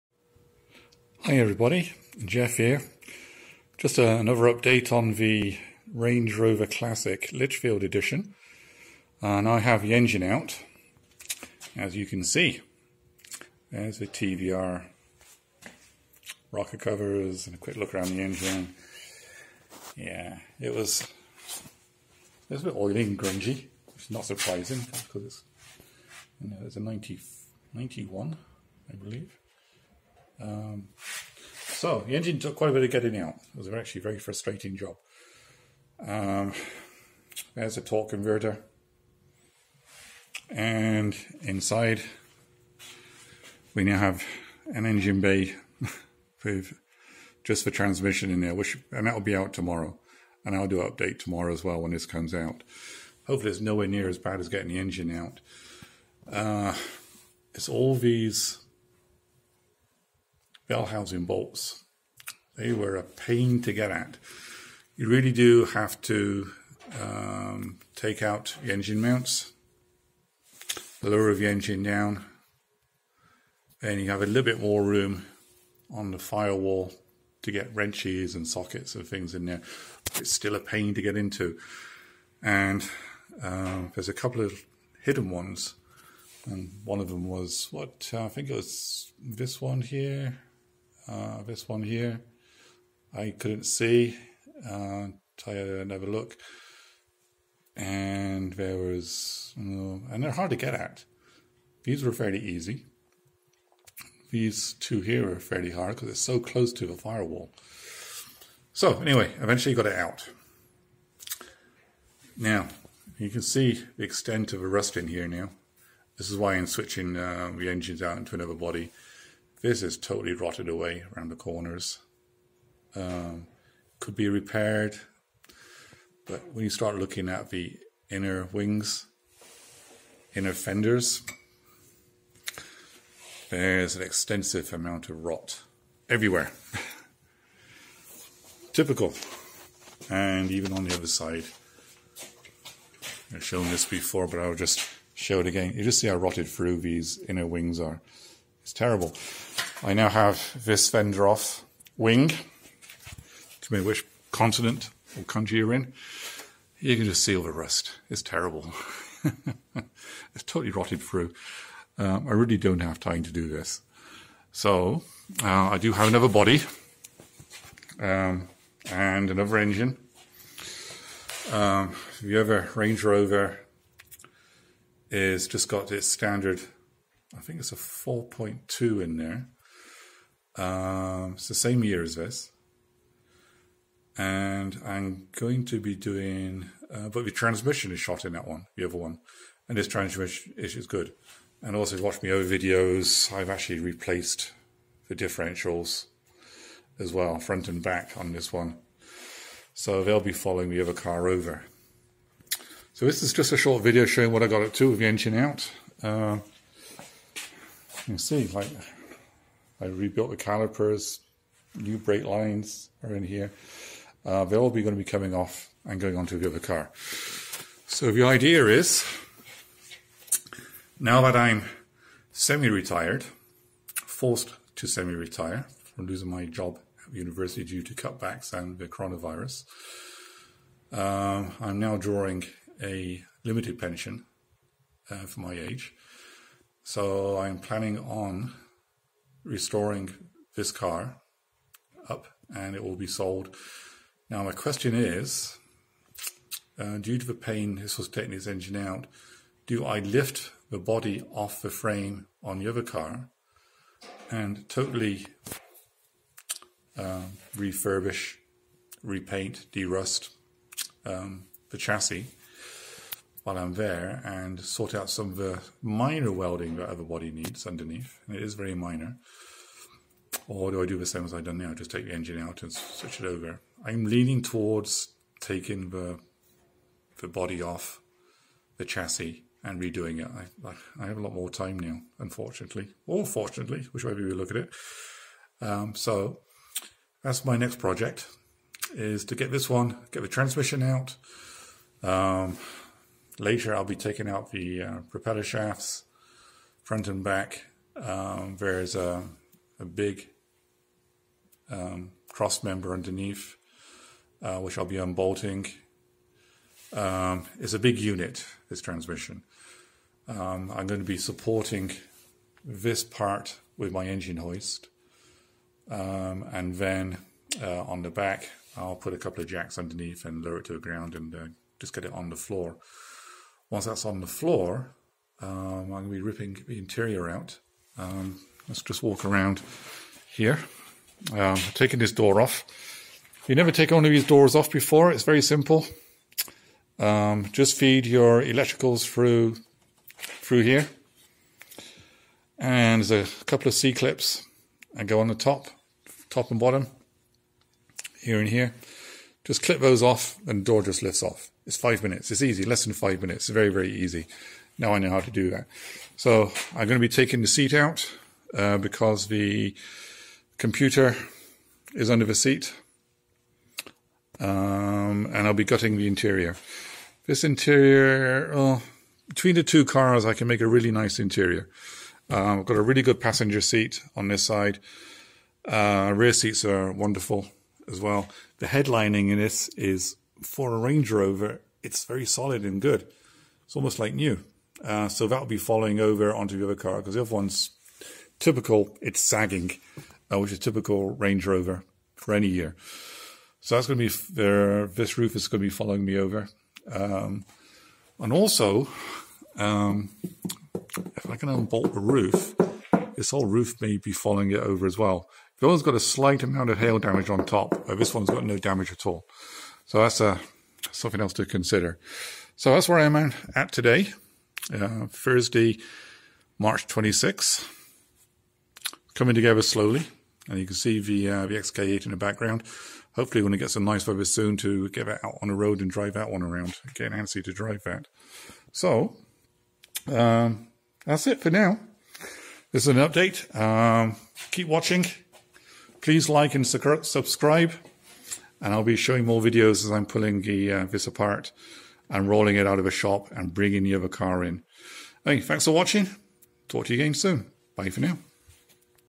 Range Rover classic Lichfield engine out
Just a quick update on where I am with the RRC Lichfield. Engine out